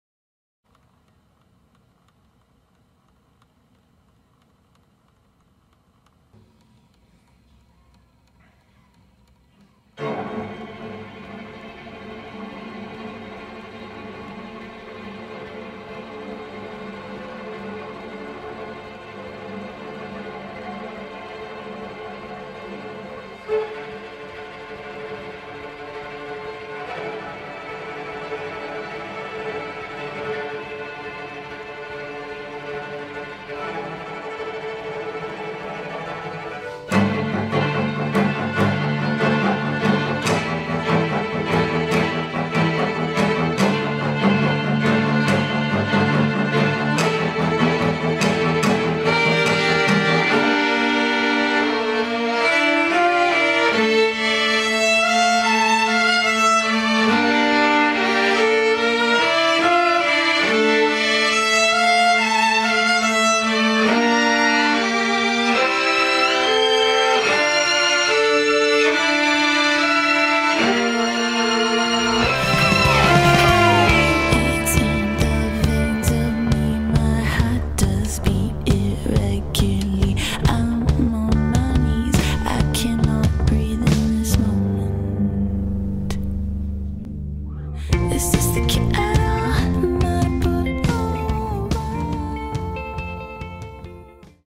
Future Brite Studio Vlog: Brite Strings
Jean Cook, Pat Muchmore and Hubert Chen (of New York's inimitable Anti-Social Music collective) recording strings for the title track.